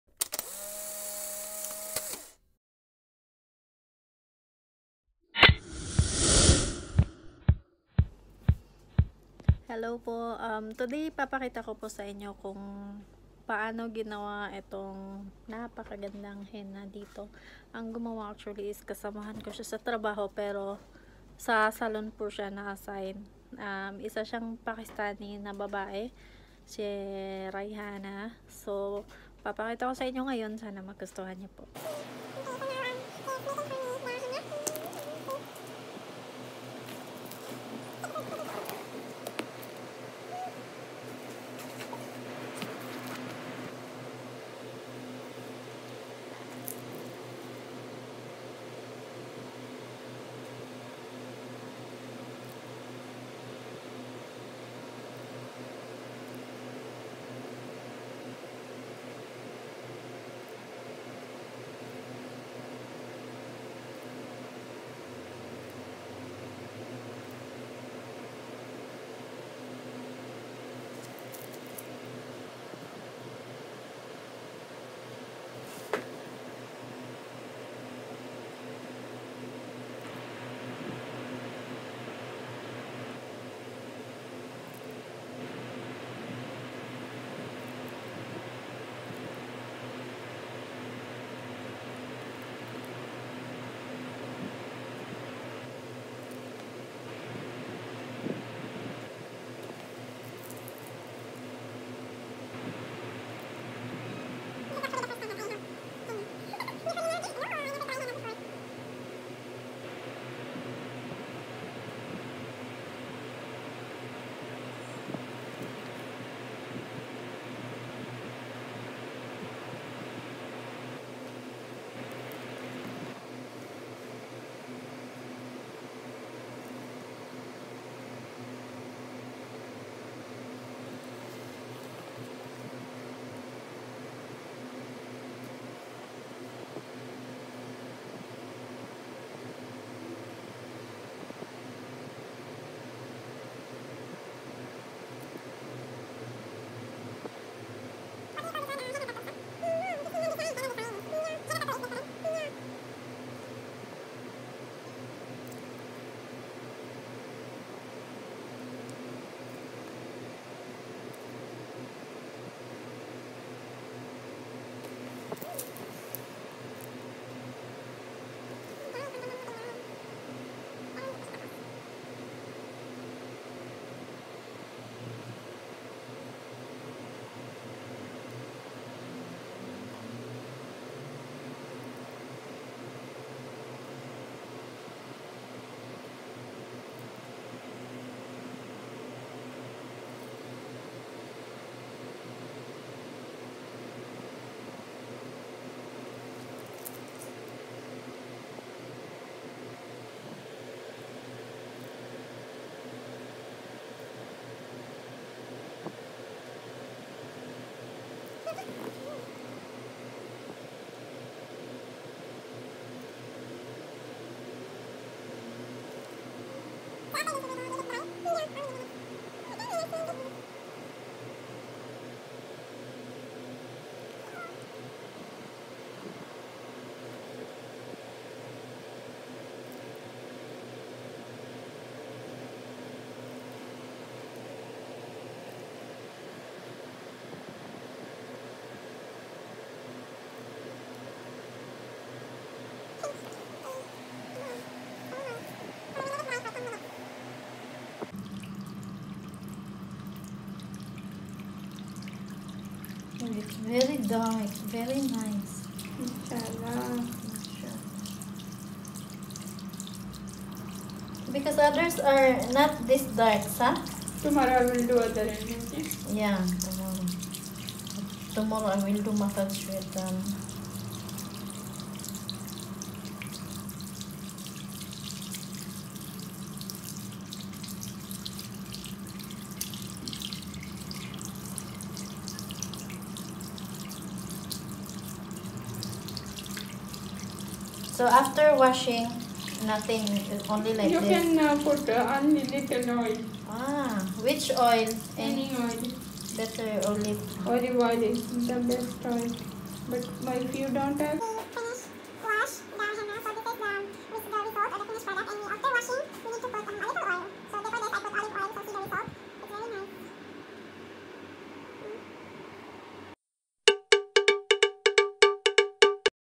Trying Henna Tattoo
This is how henna is done.. Here in the UAE, if there are special occasions, women put henna on their lower arms and hands..Guys she can do homeservice here in Al Ain for henna and other saloon services..